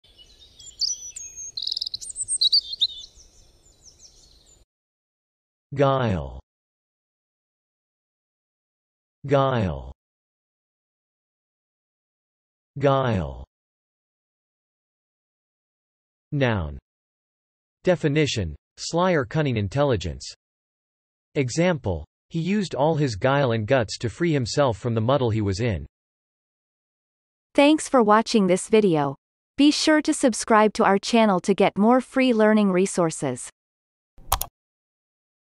How to pronounce guile - Vocab Today
The pronunciation of the word guile in Amercian accent is demonstrated in this video. If the word is from another language, such as brand name, it will be pronounced in its original language. Based on the word frequency, this is of 4 stars difficulty.

The word guile has the following meanings:
1. (noun)
Definition : Sly or cunning intelligence.
Example : he used all his guile and guts to free himself from the muddle he was in